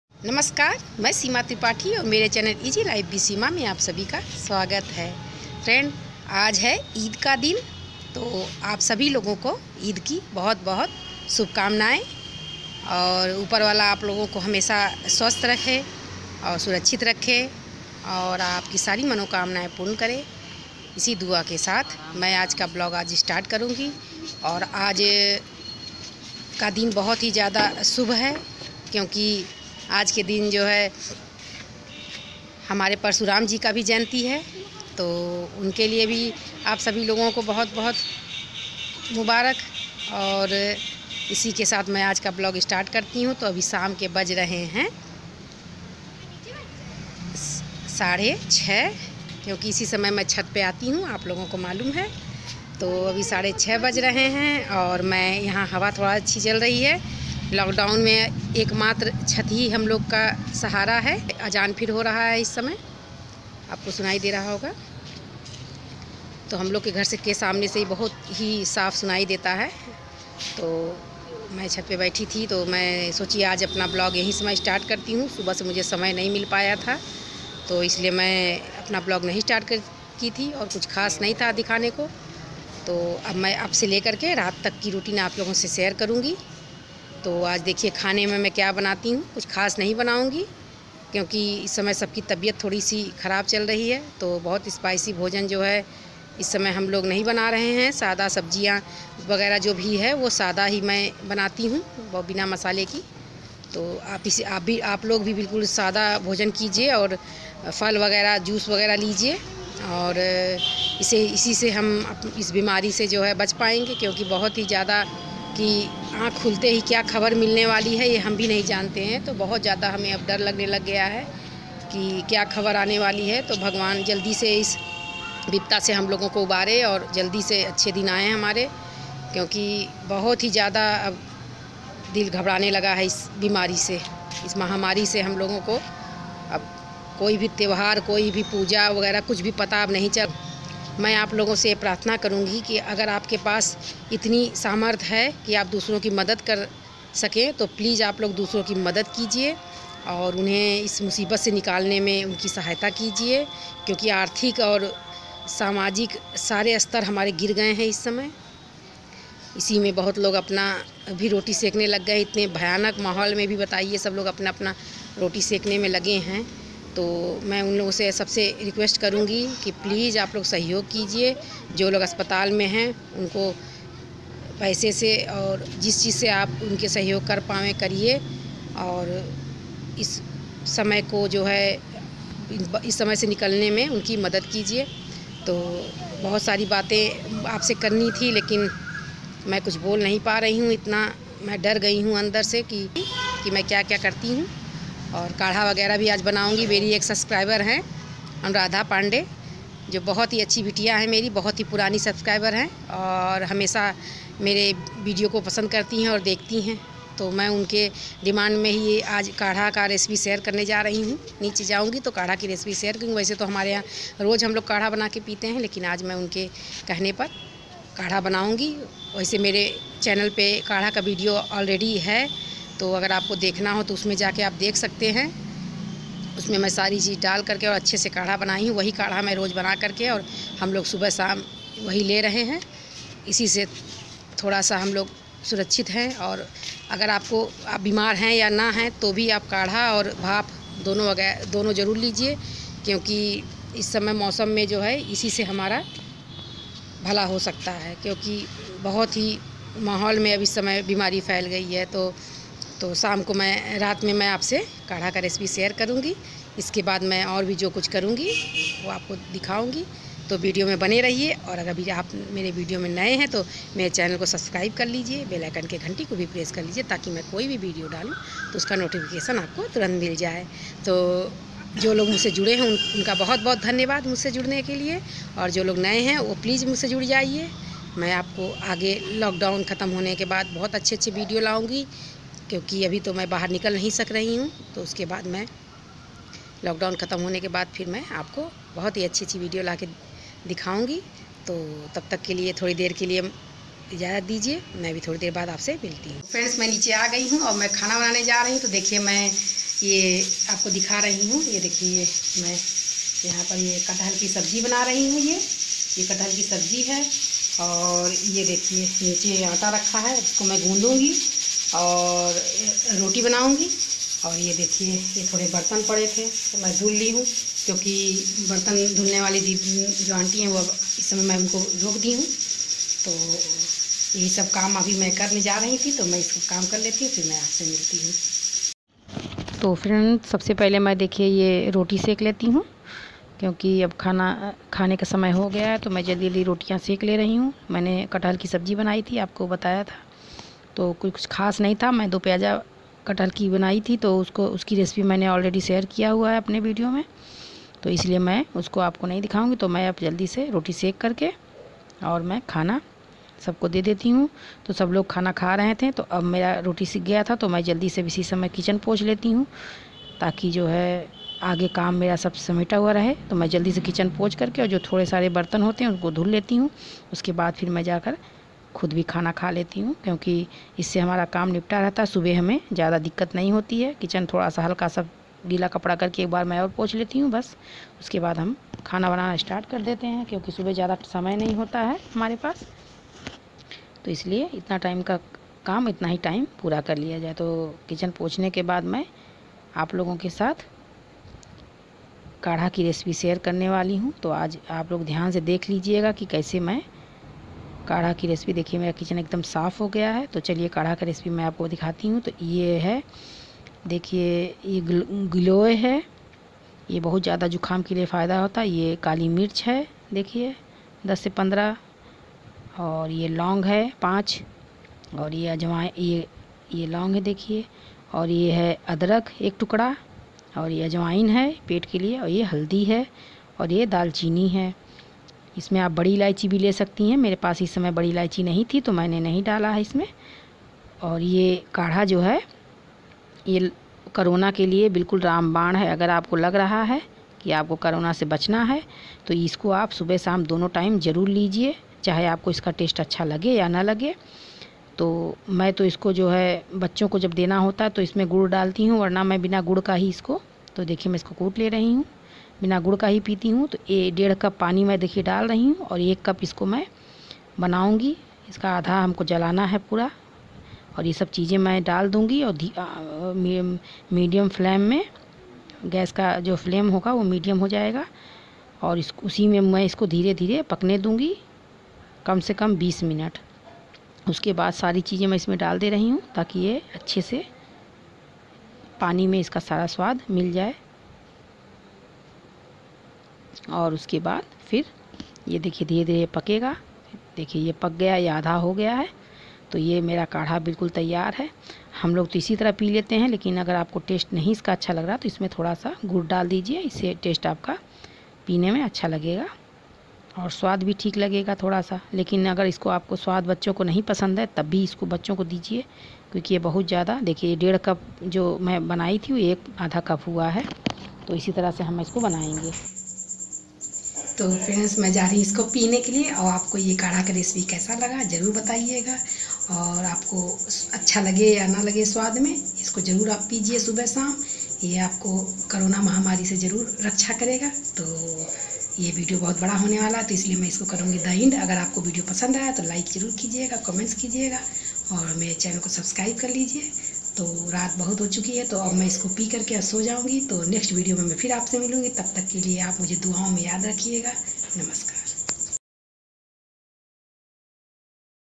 kadha banane ka Aasan vidhi Jo karona se aapko hundred percent surakshit rakhega
easylifewithSeema kadharecipe kadhibananeki Aasanvidhi#